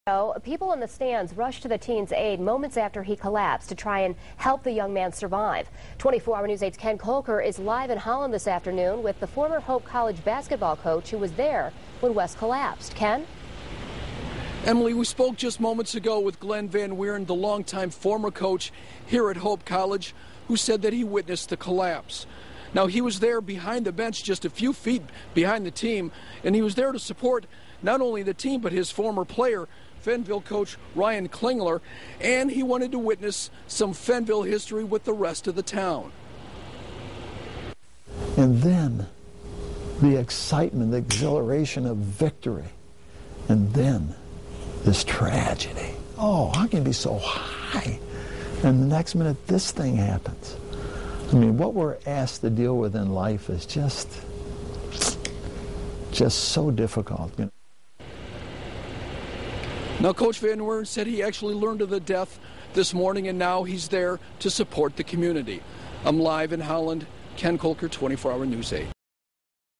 Van Wieren: Victory then tragedy
Former Hope College men's basketball coach Glenn Van Wieren was at Fennville High School boys' basketball game Thursday night when Wes Leonard scored the game-winning shot and then collapsed and died afterward.